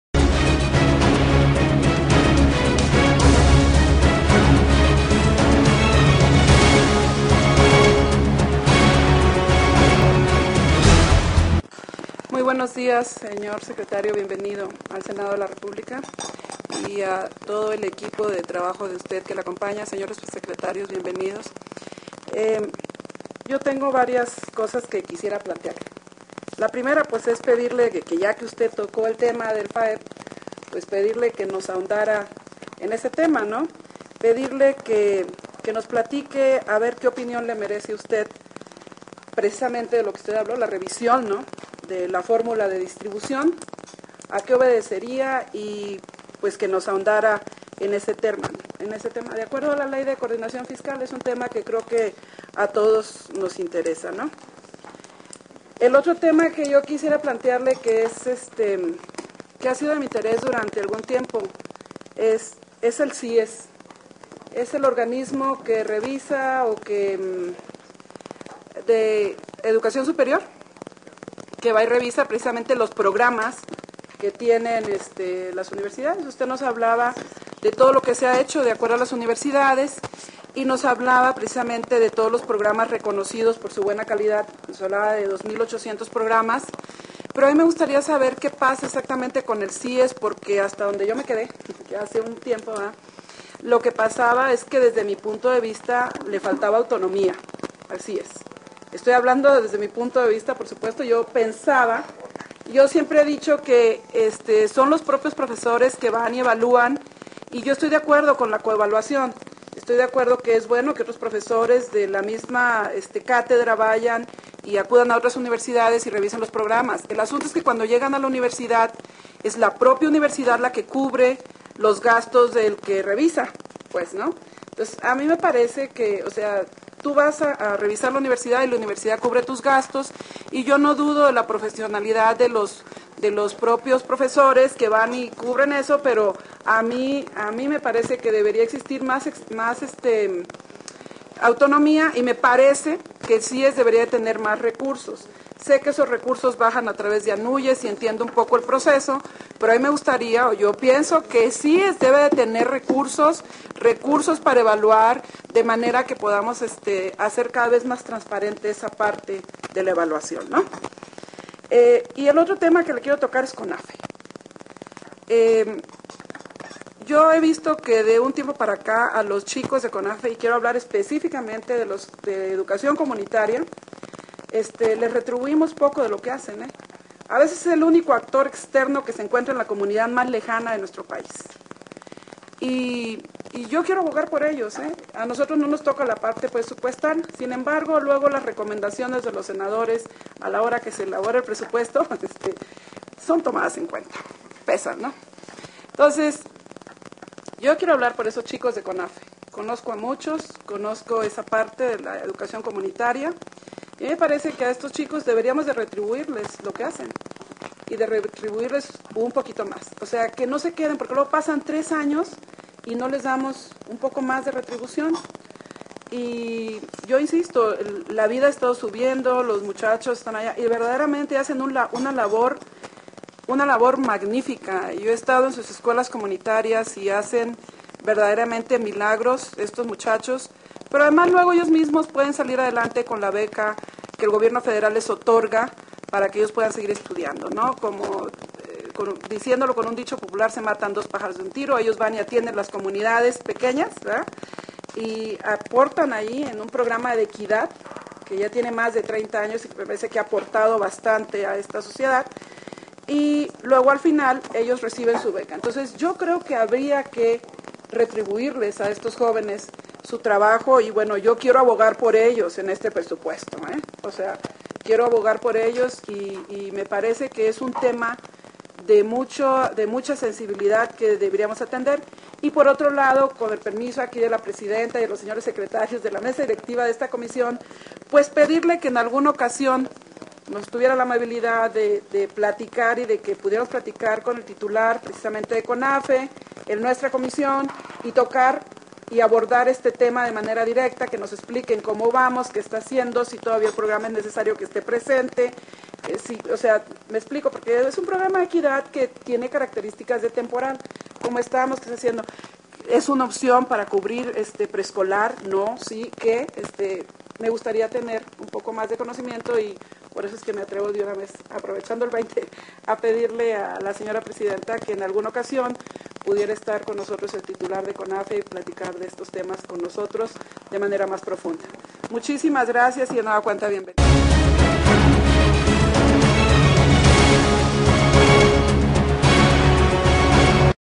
23 sept. 09 Senadora Blanca Judith Díaz Delgado
México D.F., 23 de septiembre de 2009.- Intervención de la senadora panista Blanca Judith Díaz Delgado en la comparecencia del secretario de Desarrollo Social, Ernesto Cordero Arroyo